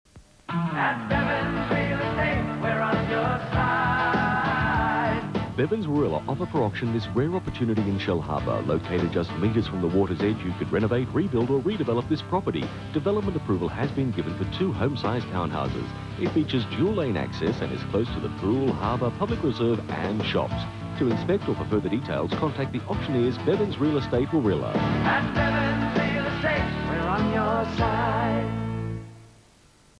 Bevans Real Estate Group Warilla - 30 Second Ad (May 1996)
Recorded off WIN Illawarra on Wednesday the 29th of May 1996.

Equipment:
Ripped from a OPUS E-180 VHS Tape recorded in LP Speed and Mono Audio.
Captured on a Panasonic DMR-BWT955 Hard Drive Recorder in XP quality. Edited and Encoded on Adobe Premiere Pro and VirtualDub2.